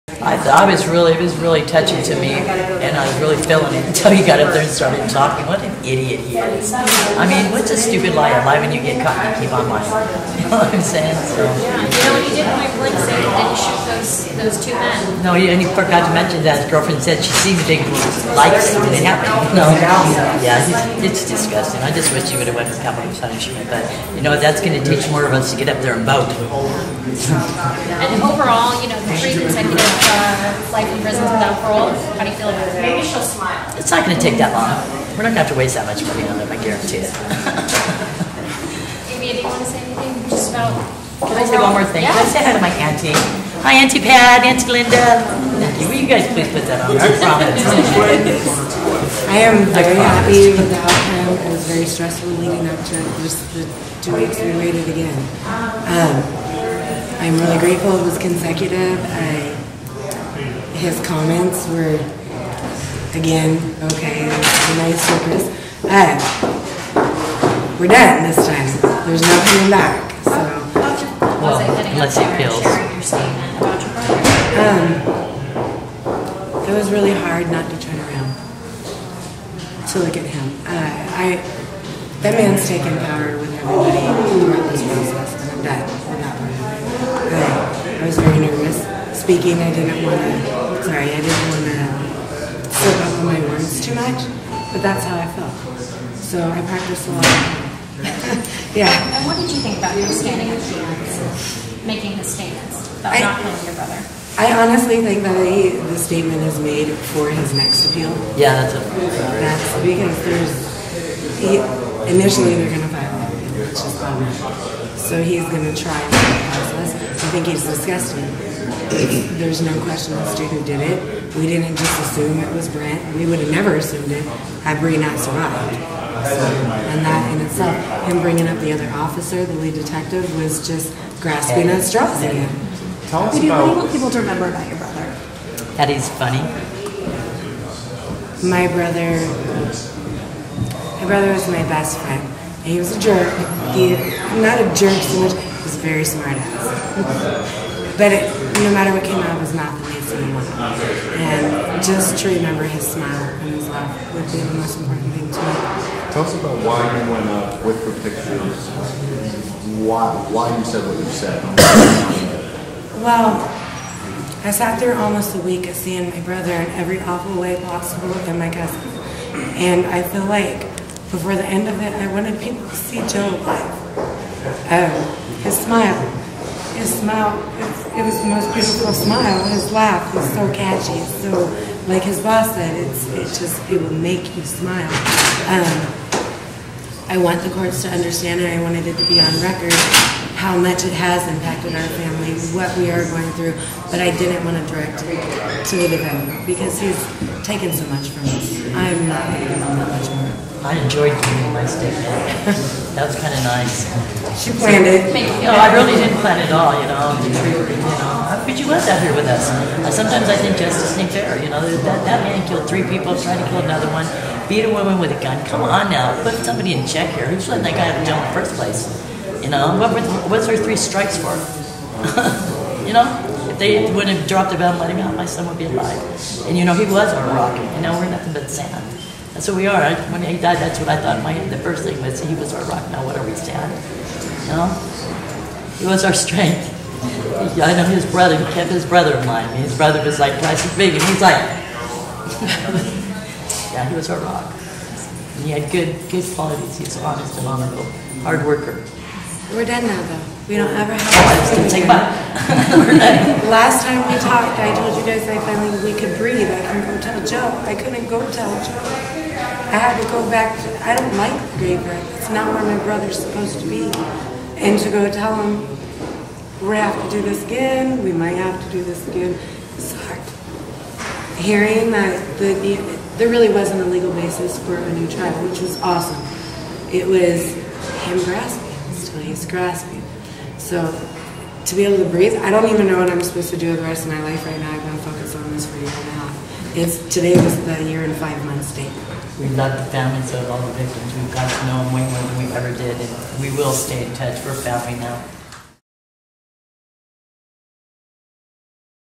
Family of victim speak after Luyster's sentencing
Brent Luyster was sentenced Friday December 15, 2017, to life in prison without the possibility of release, bringing the saga of one of Clark County’s most notorious murder cases to an end. The family of one of the victims, Jospeh LaMar, spoke following the sentencing at Clark Superior Court.